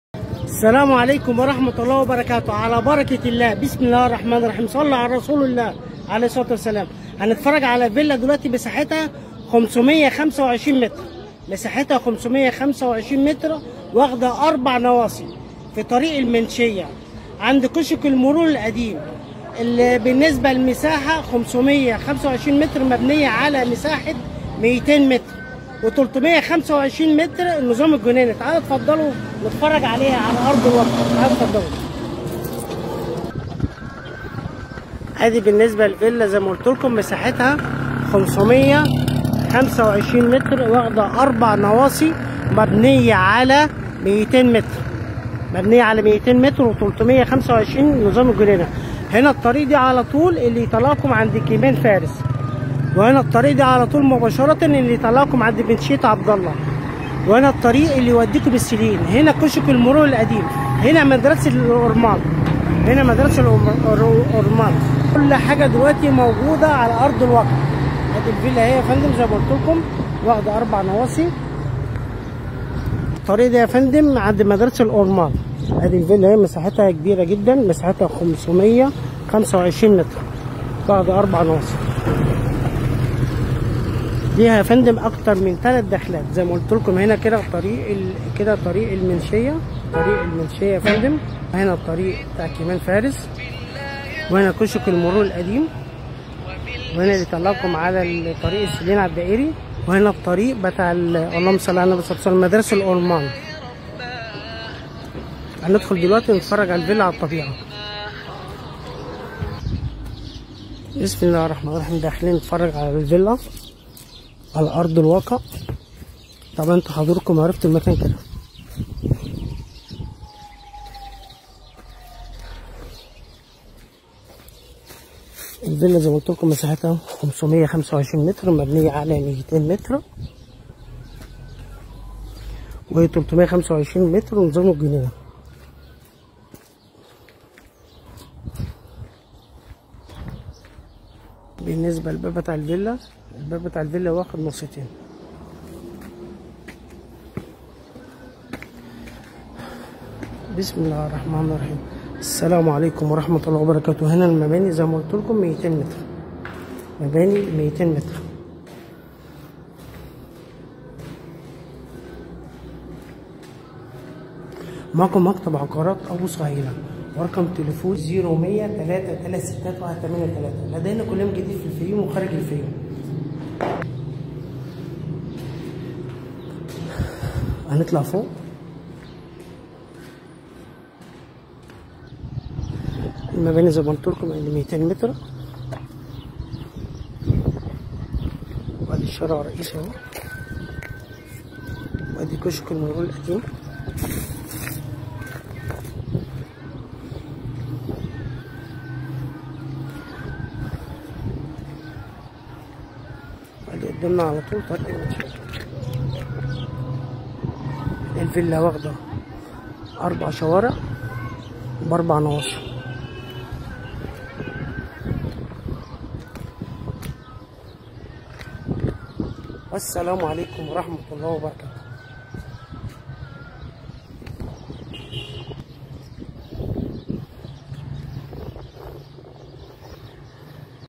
فيلا مساحة 525 متر تصلح لعمل مشروع ادارى مستشفى او او مركز طبى او بنزينه
فيلا مساحة 525 متر تصلح لعمل مشروع ادارى ( مستشفى او او مركز طبى او بنزينه او أفكار ل مشاريع كتير مختلفه ) ,,, على طريق المنشيه وقريبه من مدرسة الاورمان للاستعلام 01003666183
شقق للبيع
شقتك
شقق
عقارات
تسويق عقارى
فيديوهات صغيرة
youtube short
youtube shorts
shorts
short